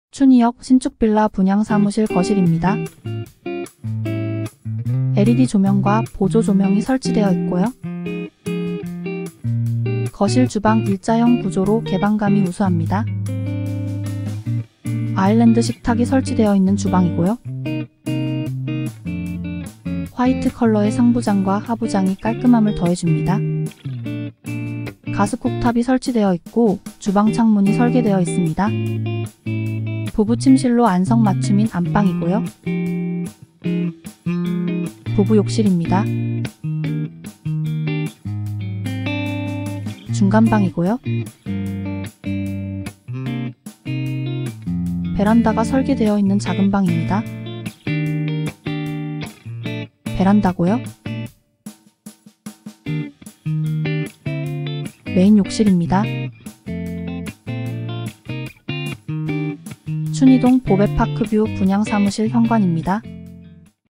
춘의역신축빌라 춘의동 보배파크뷰 분양 지하철 7호선 역세권 우수한 위치 가격까지 저렴한빌라
서울 경기 인천 신축 분양사무실 모델하우스 방문예약

집방 주택 의 모든 것 홈룸하우징
home room housing